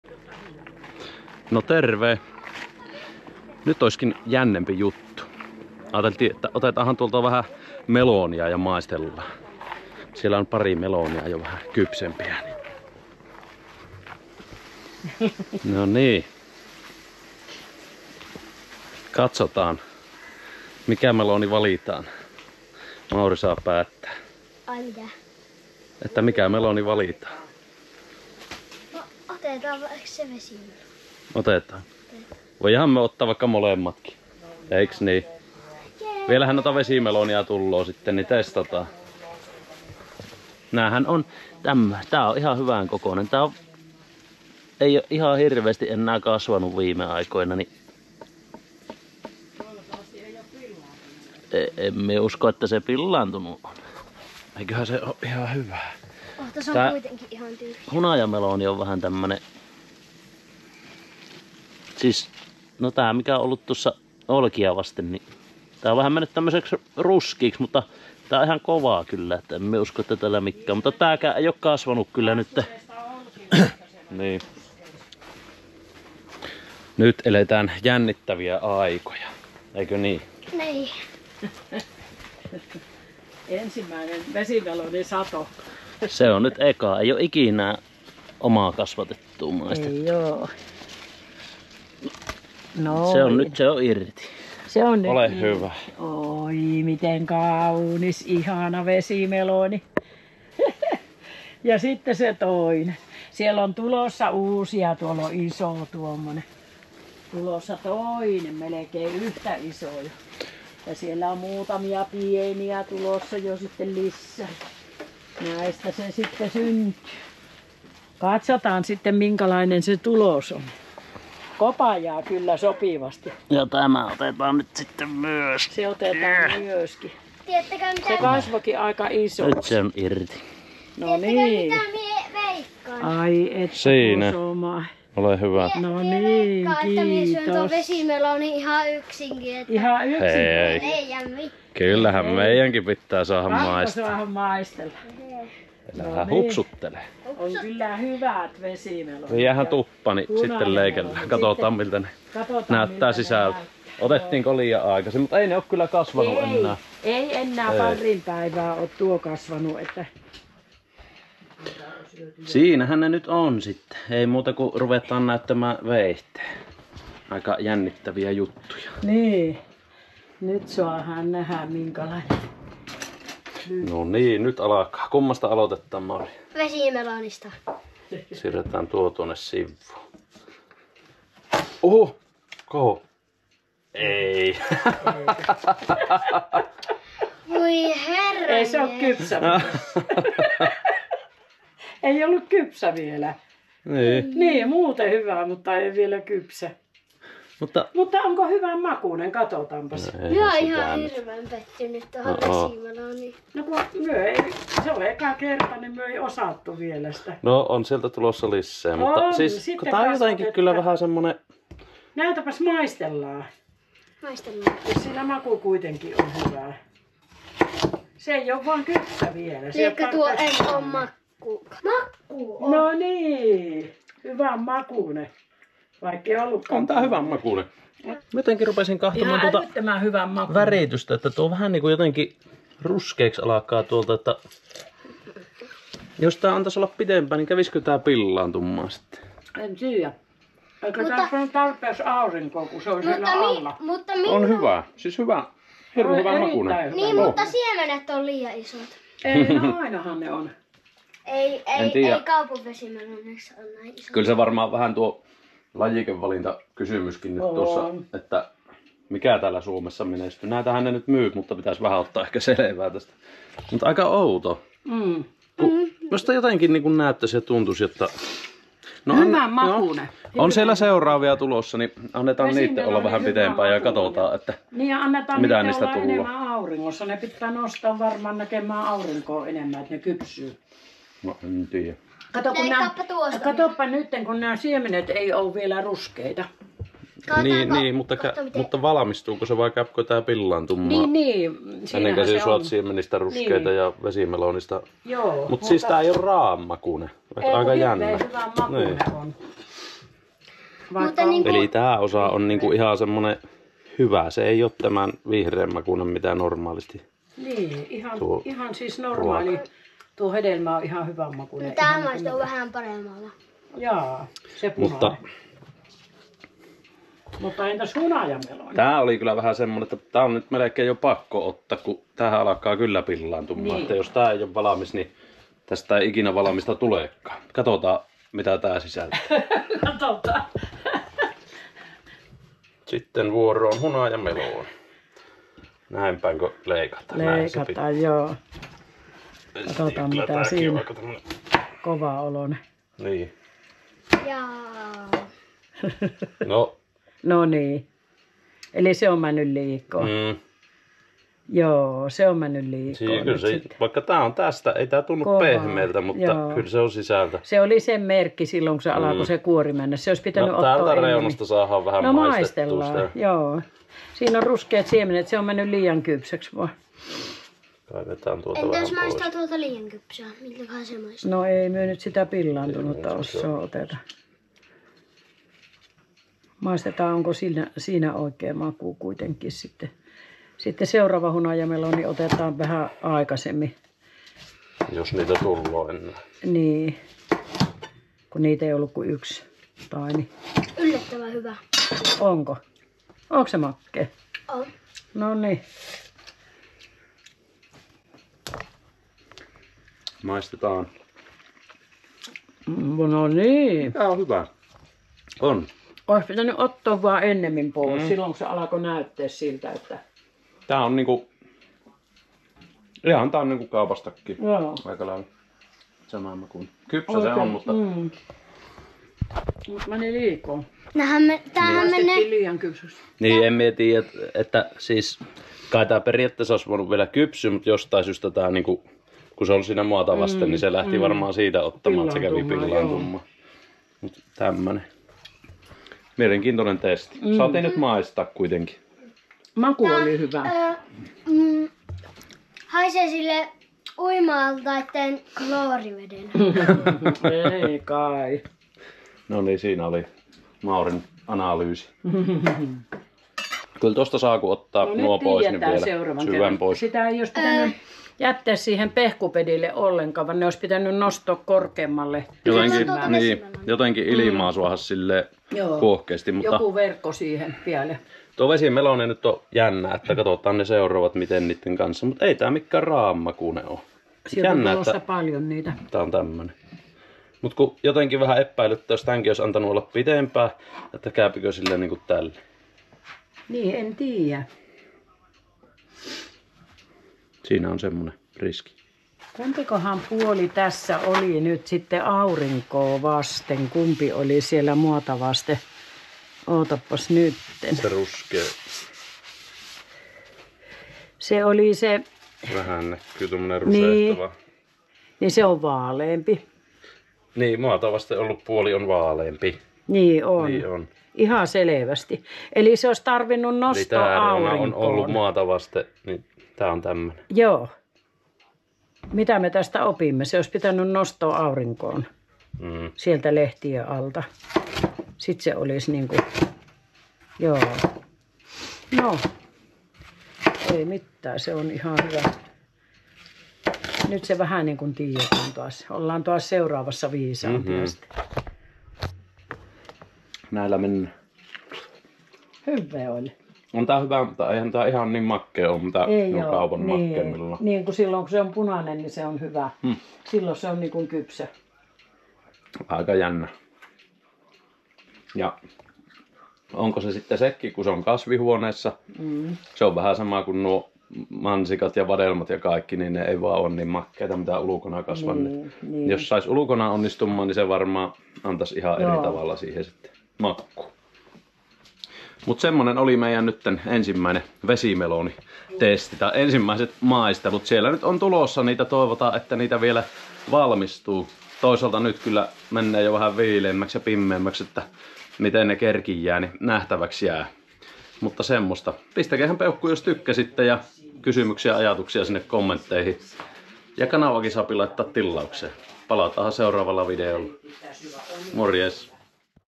Kuinka onnistui melonien kasvatus? Nyt maistetaan!
4.8. Nyt se nähdään, kuinka ensimmäiset kasvihuoneessa itsekasvatetut vesi- ja hunajamelonit ovat onnistuneet. Haetaan melonit kasvihuoneesta keittiöön ja paljastetaan lopputulos. Sekä vesimeloni, että hunajameloni yllättävät maistajaporukan.

Vlogi sääriamputoidun maanviljelijän elämästä Pohjois-Karjalassa, työstä, harrastuksista ja haasteista.